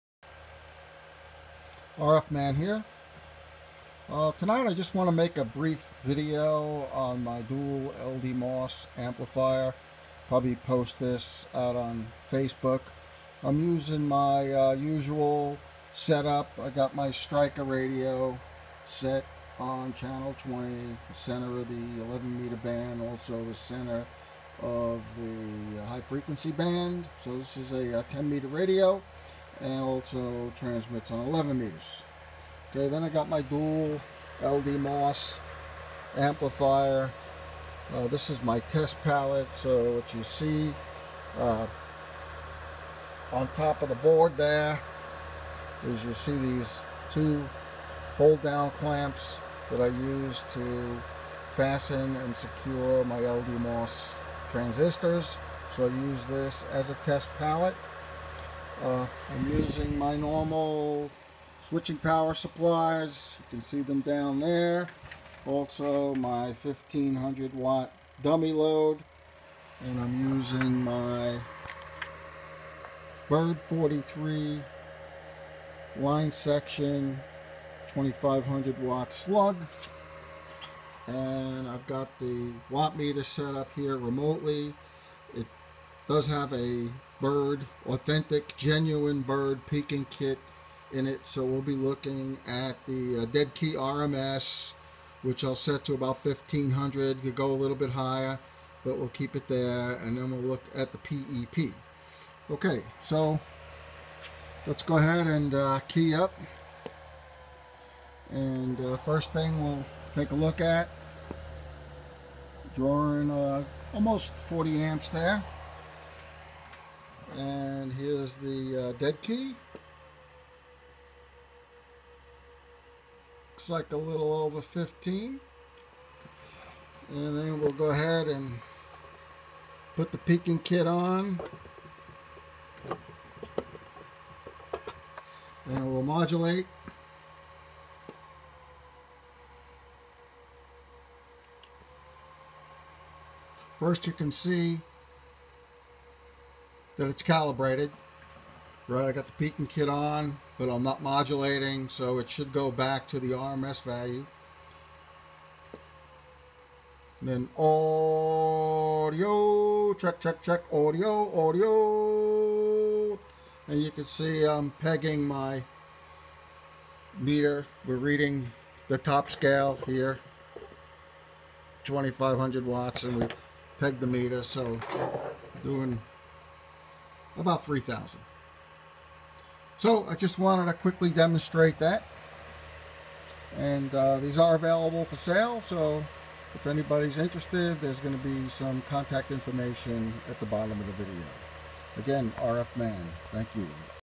RF Man Demos Dual LDMOS Amplifier with the Lastest Improvements.
This video shows the RMS and PEP Power output of the dual LDMOS amplifier with the latest changes and improvements.